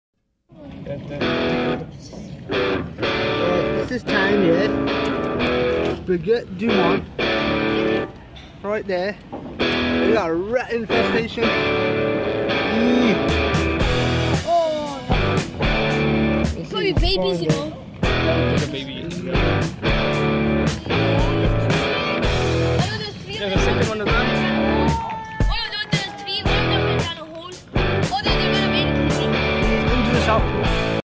Dirty rats and mice in birmingham town centre
Bugette du monte birmingham town centre just horrible if you get raisan in your food double check lolololol